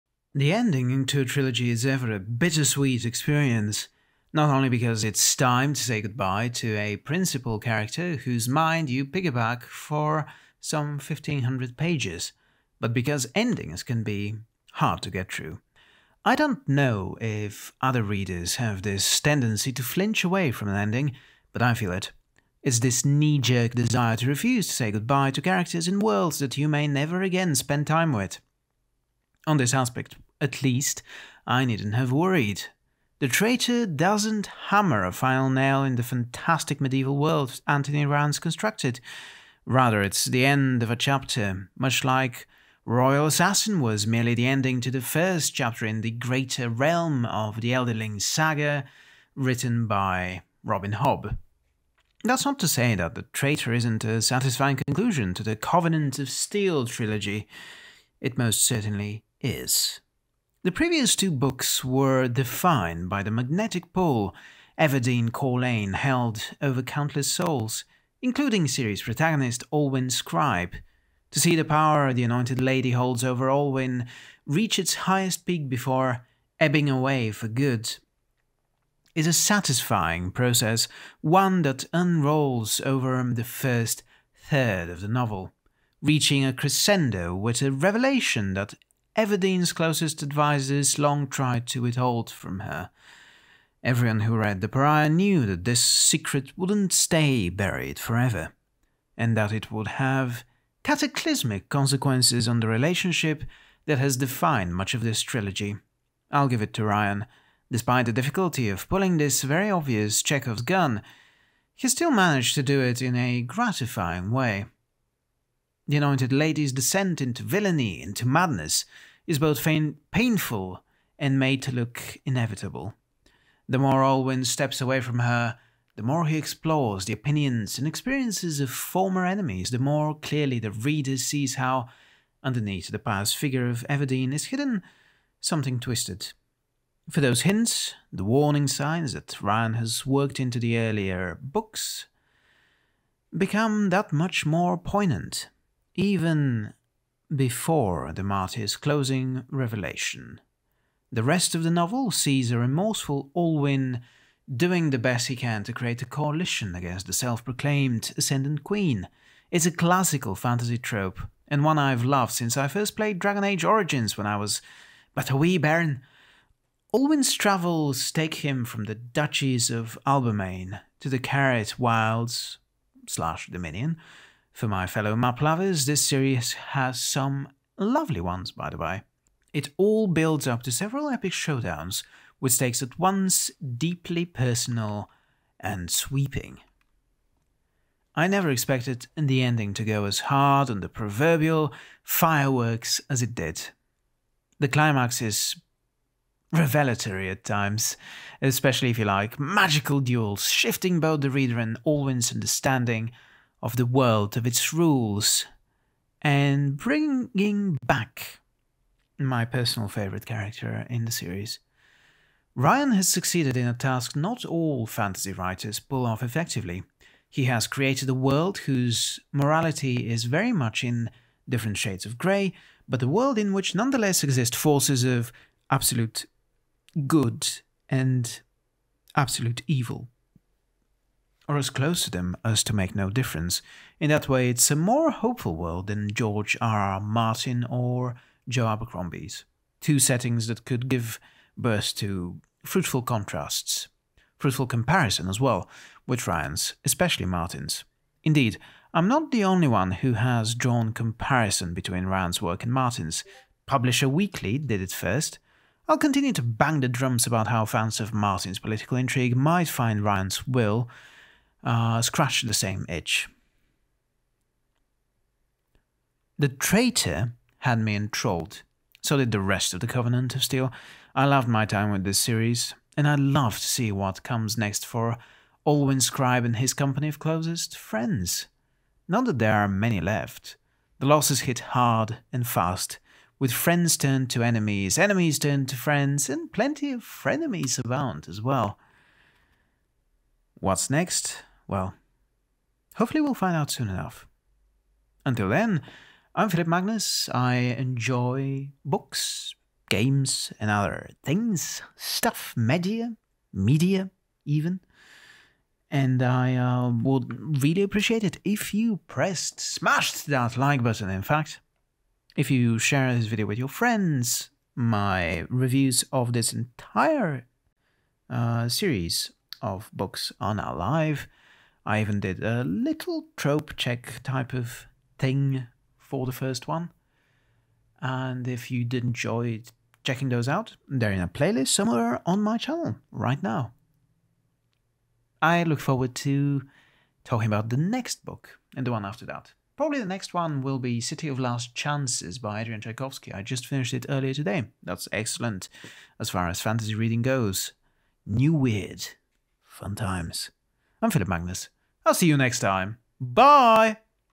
The Traitor by Anthony Ryan - Book Review | A Fitting Close to the Covenant of Steel
The Traitor by Anthony Ryan concludes the excellent Covenant of Steel trilogy! Spoilers for the previous books, but what do you expect for a third book in a series?